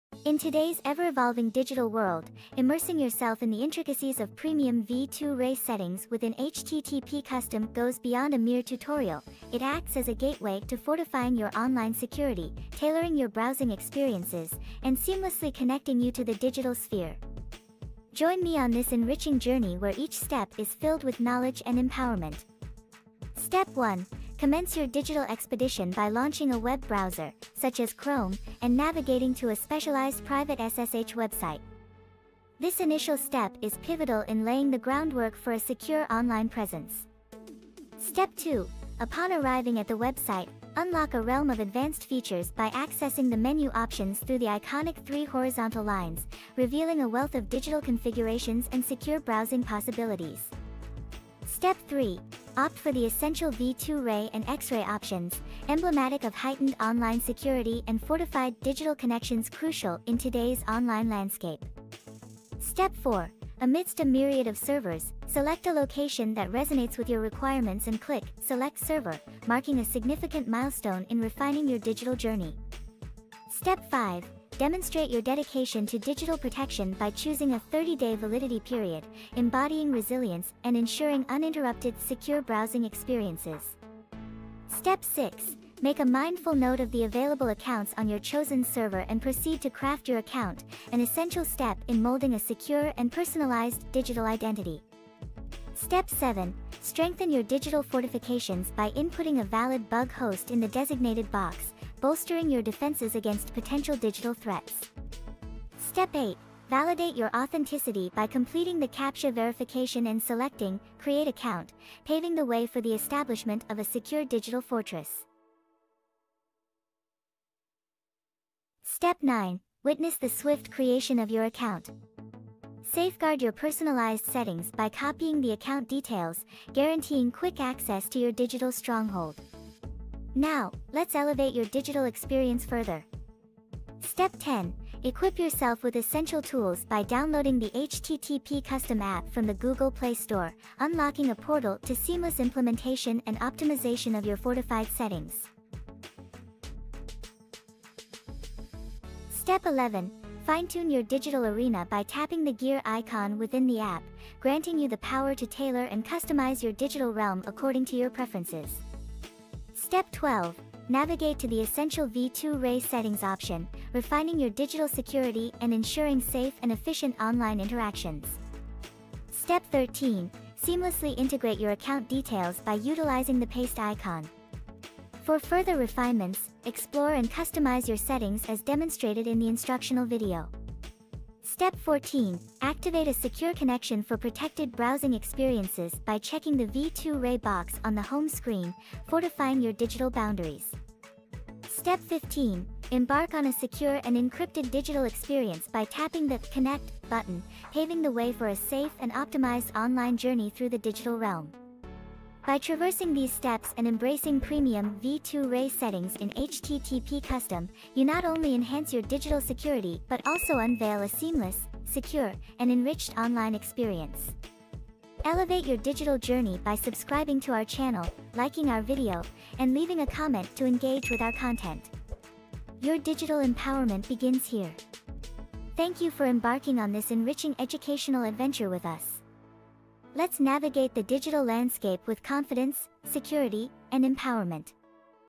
HTTP Custom Premium V2Ray Settings: A Step-by-Step Guide
This tutorial offers a comprehensive guide on configuring premium V2Ray settings in the HTTP custom app, focusing on boosting your online security and enhancing browsing experiences. It takes you through the process of selecting a secure server, creating a personalized account, and configuring essential settings for encrypted browsing. The steps include choosing a server, validating the account, and integrating the details into the HTTP custom app. By activating secure connections, users can navigate the digital world with fortified protection and optimized performance.